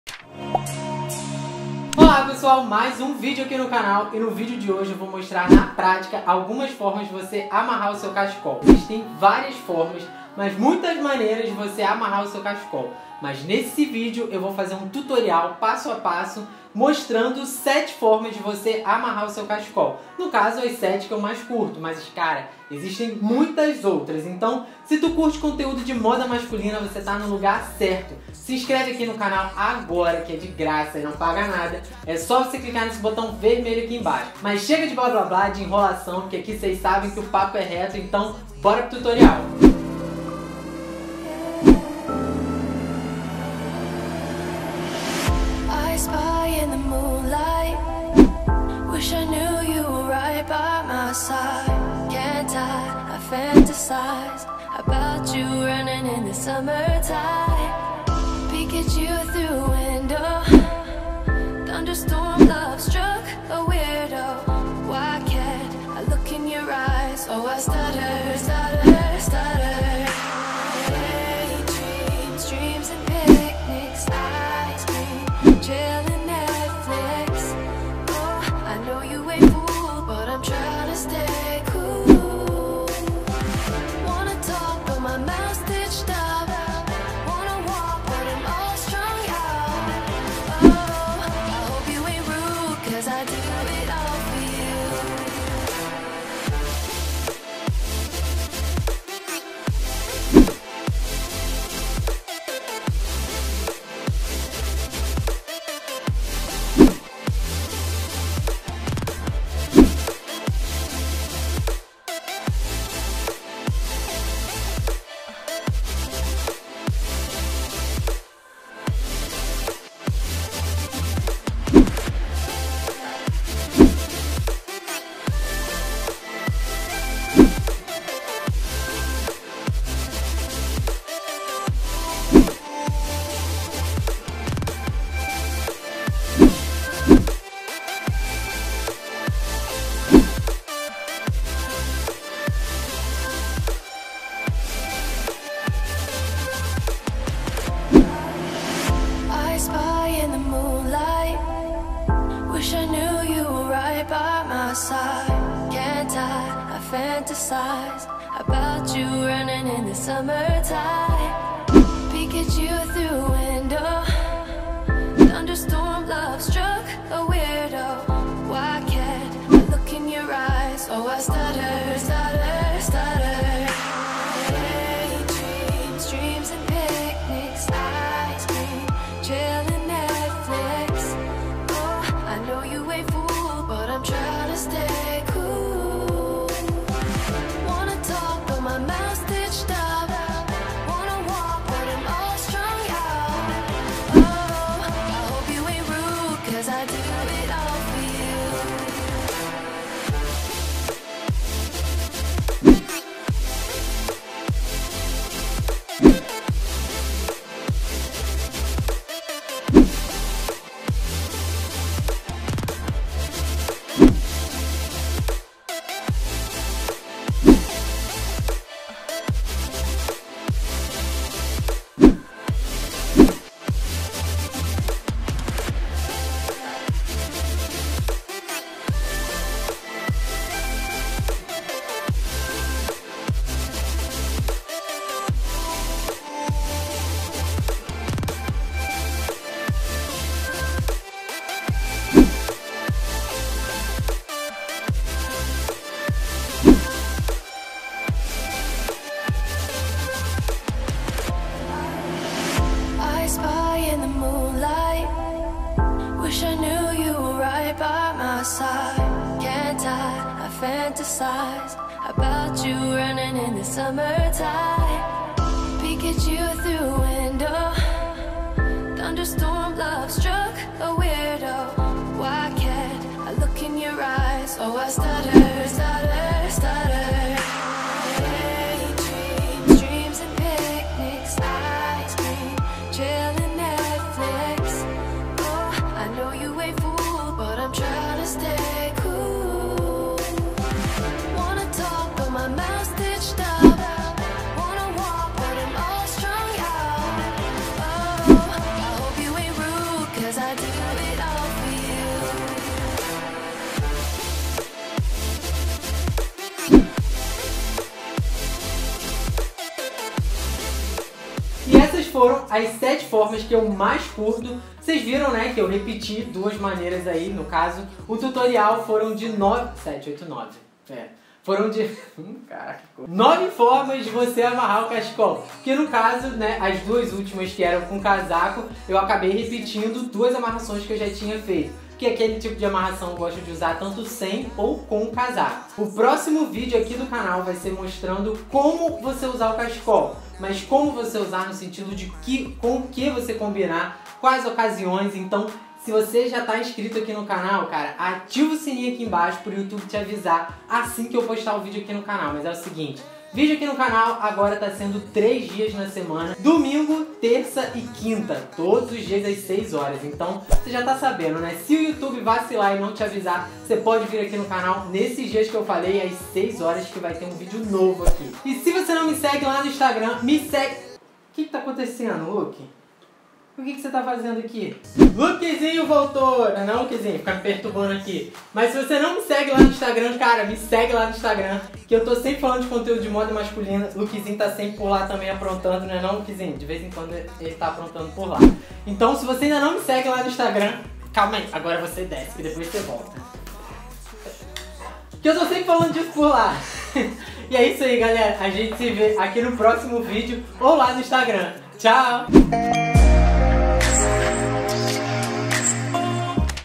Tutorial: como usar CACHECOL MASCULINO
Tá na dúvida de como dar um nó ou amarrar o seu cachecol? Então se liga nesse tutorial rápido e fácil aqui.